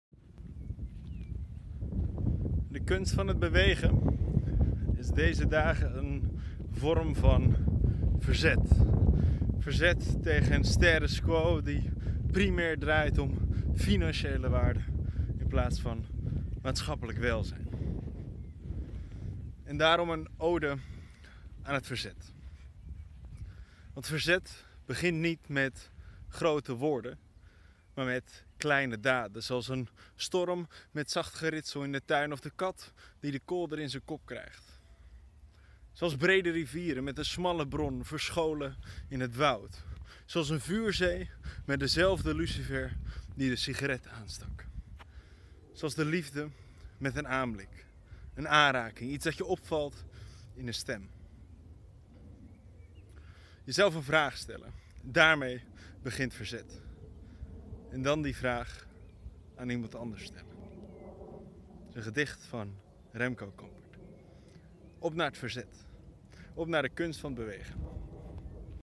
Een ode aan De Kunst van Bewegen door Werner Schouten (Impact Economy Foundation)
Werner Schouten (Impact Economy Foundation) geeft aan de hand van een gedicht een ode aan het verzet. Verzet tegen een status quo die primair draait om financiële waarden in plaats van maatschappelijk welzijn. Op naar De Kunst van Bewegen.